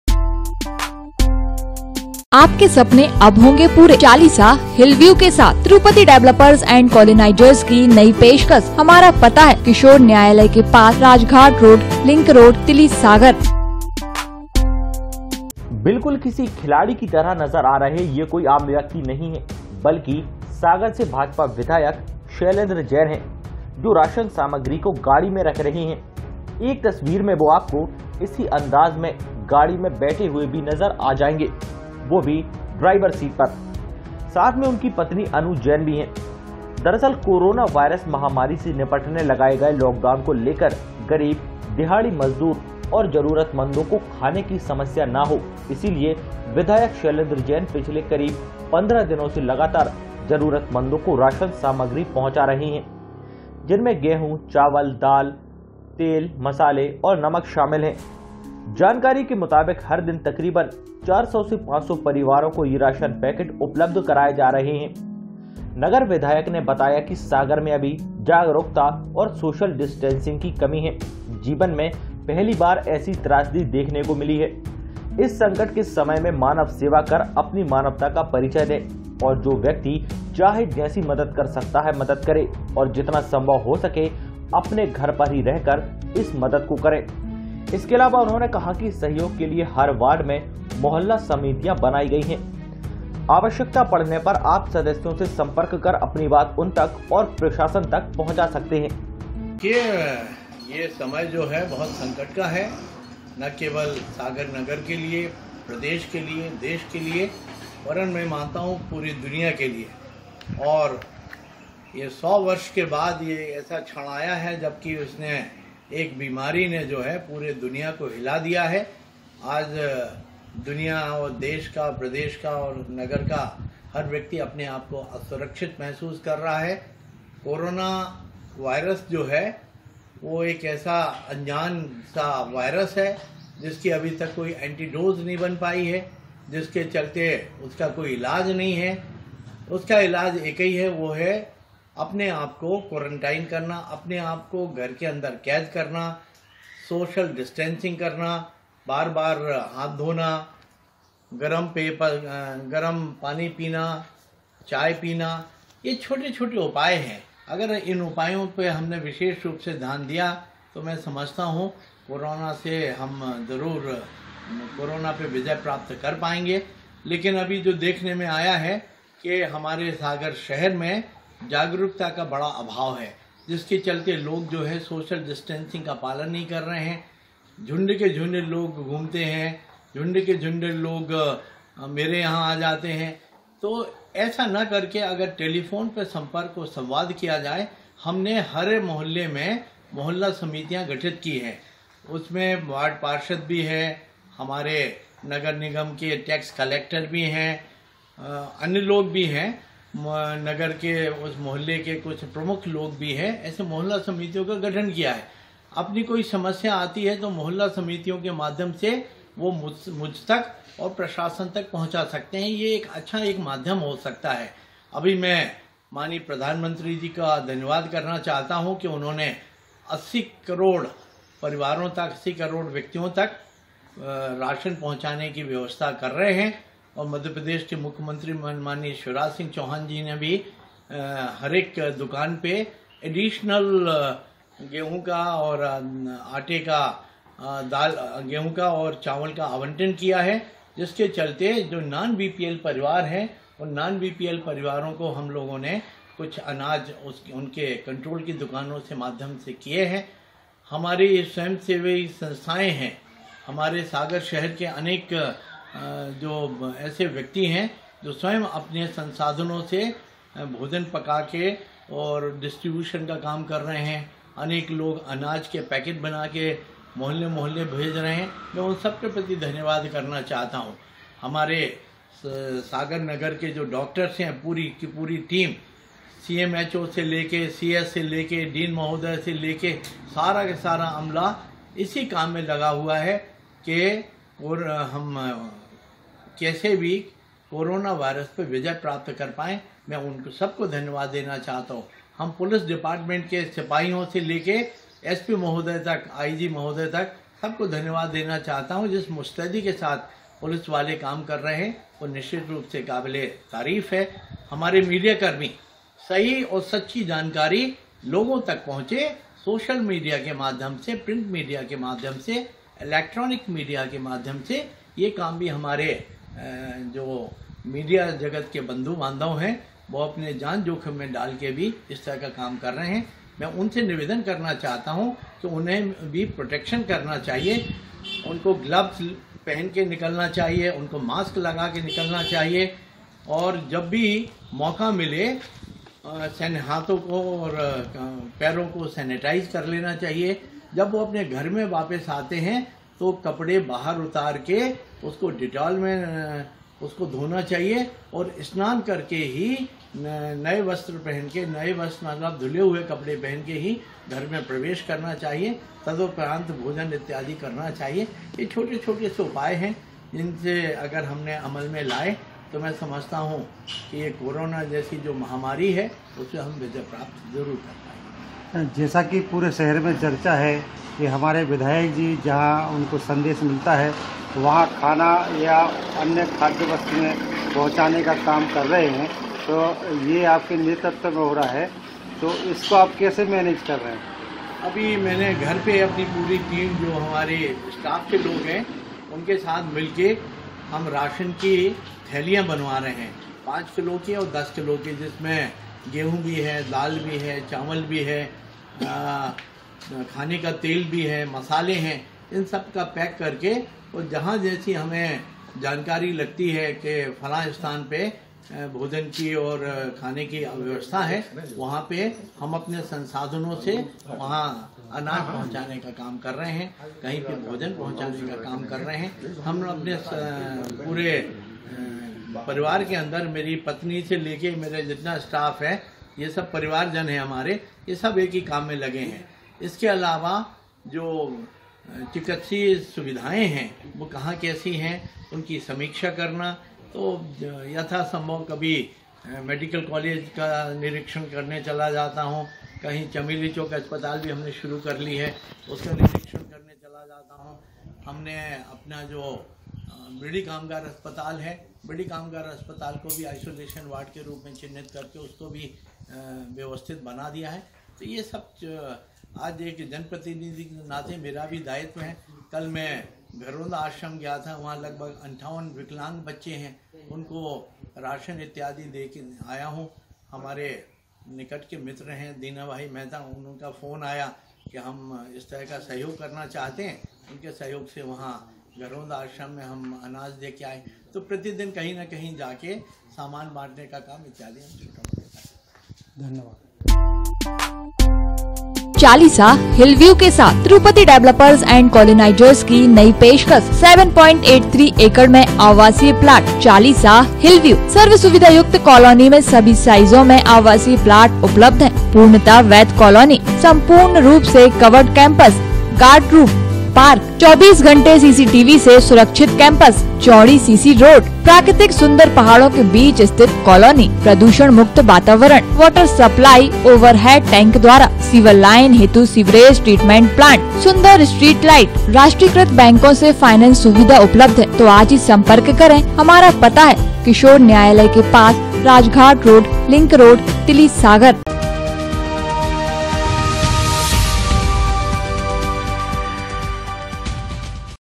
500 परिवारों तक रोज राशन सामग्री पहुंचा रहे सागर विधायक || SAGAR TV NEWS
500 परिवारों तक रोज राशन सामग्री पहुंचा रहे सागर विधायक  || SAGAR TV NEWS ||